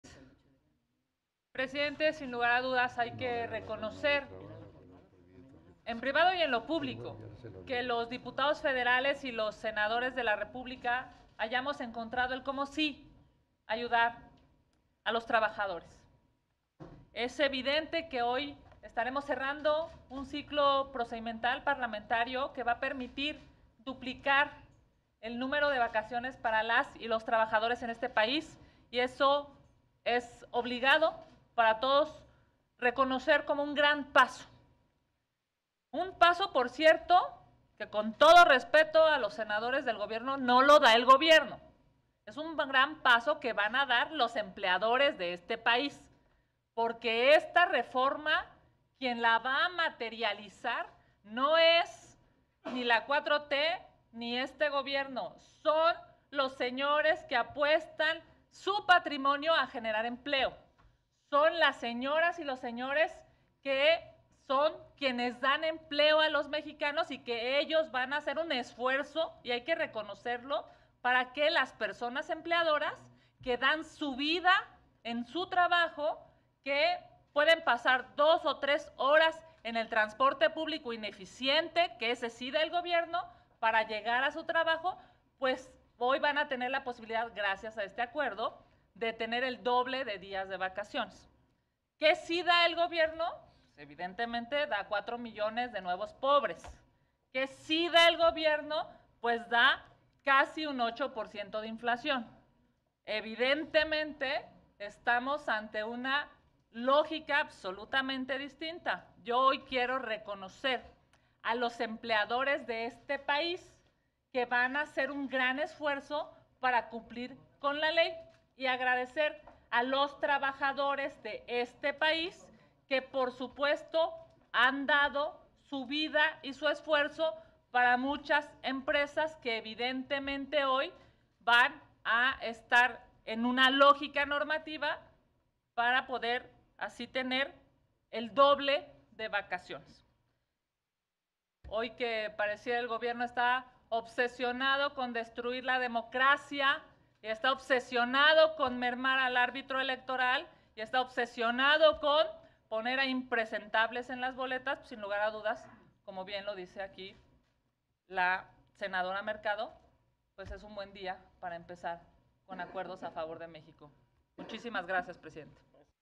Senadora Kenia López Rabadán: 12 días consecutivos de vacaciones para los trabajadores
14 de diciembre de 2022.- Intervención de la senadora Kenia López Rabadán en la reunión de las comisiones unidas de Trabajo y Previsión Social, y de Estudios Legislativos Segunda para referirse al dictamen de las Comisiones Unidas de Trabajo y Previsión Social y de Estudios Legislativos, Segunda, recaído a la Minuta con Proyecto de Decreto por el que se reforman los artículos 76 y 78 de la Ley Federal del Trabajo, en materia de vacaciones.